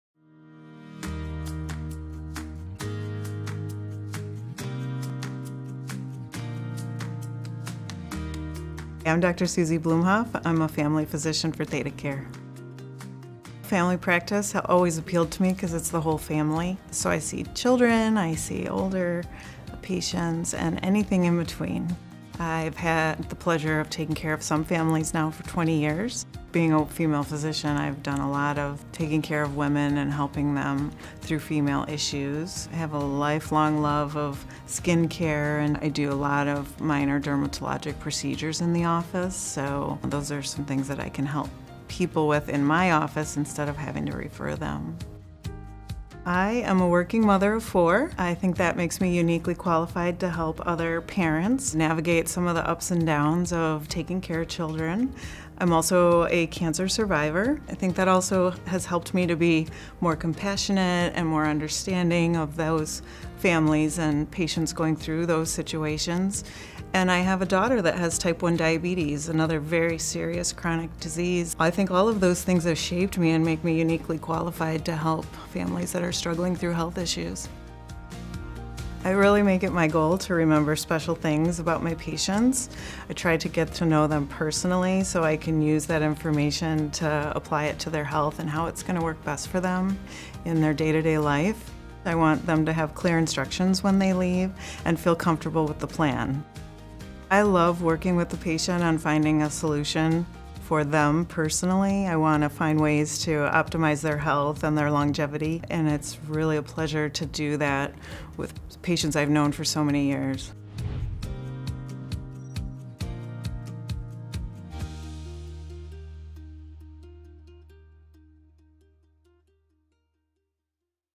Meet Dr. Suzanne Bloomhuff - Family Medicine
Meet Dr. Suzanne Bloomhuff, M.D. who specializes in Family Medicine in Menasha, WI.